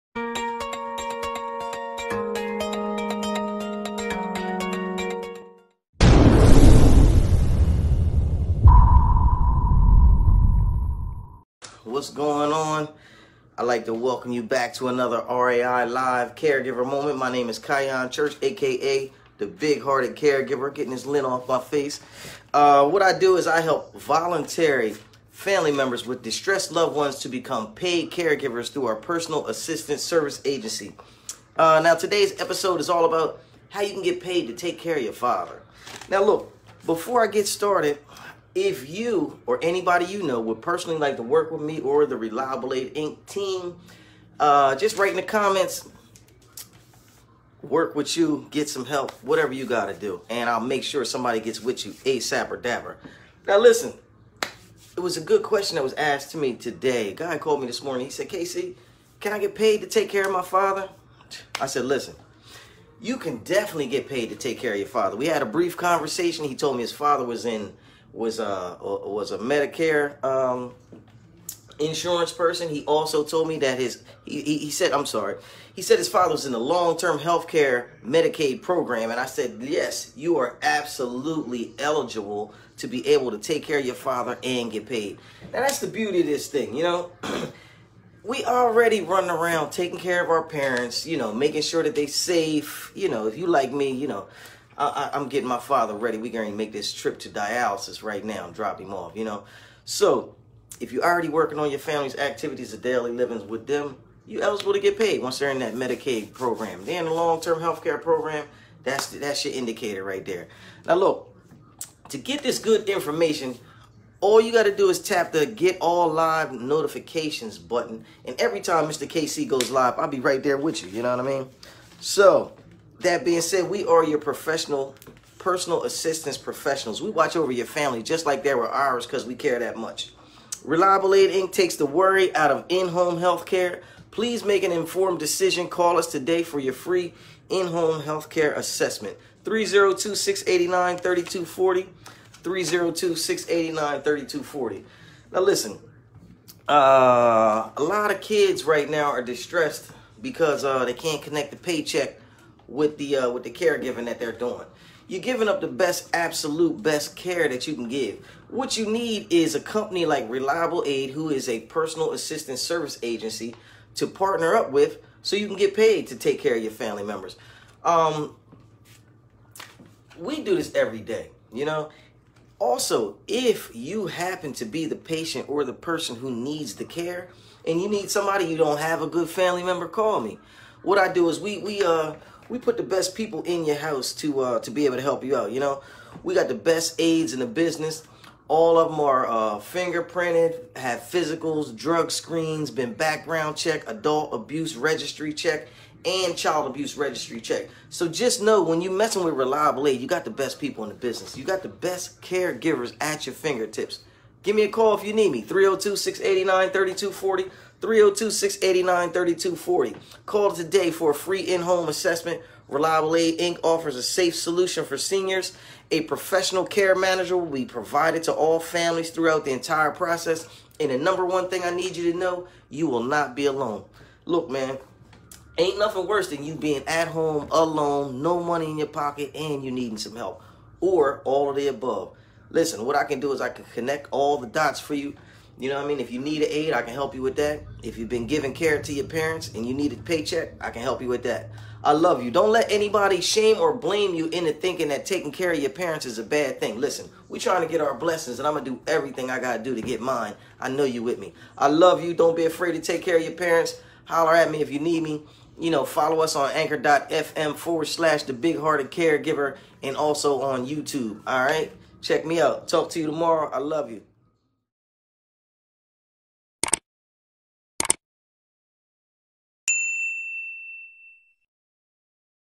Can I Get Paid For Taking Care Of My Father? 🧐￼
If you are taking care of your father and you are not sure if you can get paid taking care of him, this podcast is for you. Listen to it to the end to find out more.

If your father has Medicaid & is in the Long Term Health Care Program she is definitely qualified for a Reliable Aid! 😃

by a stranger because the family didn’t have the time or money to take care of them at home.

Everyone needs to make a living, and providing care can be very time consuming, making a decent paying job just out of reach for most volunteer
Family caregivers.

These "activities of daily living" include but are not limited to ambulating (or helping them move from one place to another), transferring (or helping them move from a bed to stretcher, a bed to a wheelchair, a wheelchair to s chair, a wheelchair to a toilet, or vice versa.),

And I know what you’re probably thinking:

If it applied to me, i would have already herd about it.  The problem is that many people believe that these caregivers need specific training or certifications in order to do the job and exclude themselves

provider and we hire family members
to take care of their loved ones that need care.

The social & physical support you've
been providing so far would be

This is important because personal assistant services do not require physician's orders, or the judgement
and skills of a licensed nurse or professional.

This means family members like your-self
qualify to become paid caregivers for
companies like Reliable Aid Inc!

The Reliable Aid team and I will walk you
through the entire process of getting your loved-one qualified for coverage, and then hiring you to take care of them!

So If your ready to start getting paid for
guide you through the process.